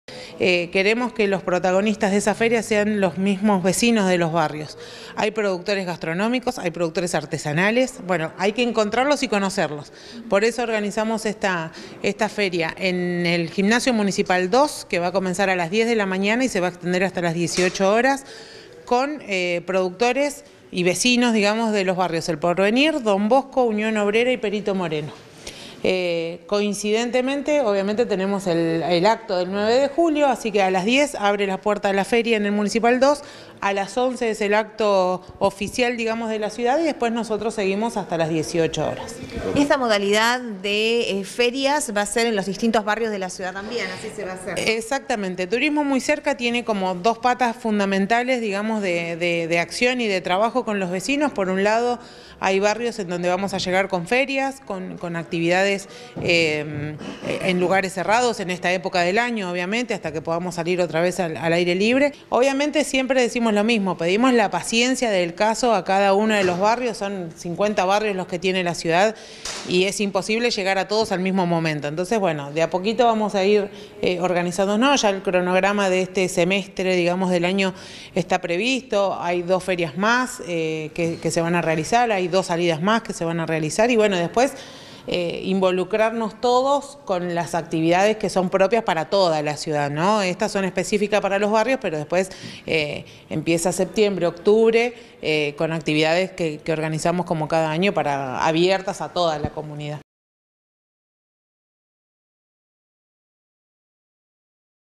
Actividades por el 9 de julio: productores barriales participarán de la feria en el Gimnasio N° 2
Nota: Mariana Filocamo, comunicación de la Secretaría de Turismo de Puerto Madryn.